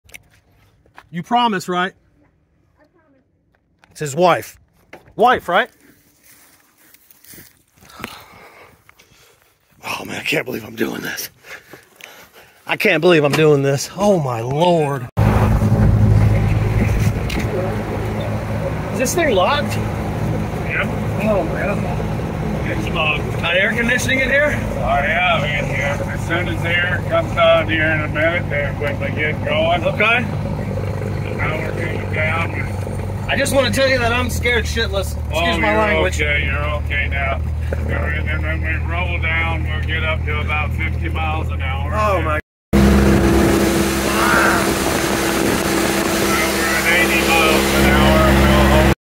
Randy Rhoads flight path and crash location.
Randy Rhoads flight path. the owner of the airplane and bus lines where Randy crashed took me up for a flight around his property. sorry for the language, scared the cr$p out of me! He was a great pilot though, calmed me right down.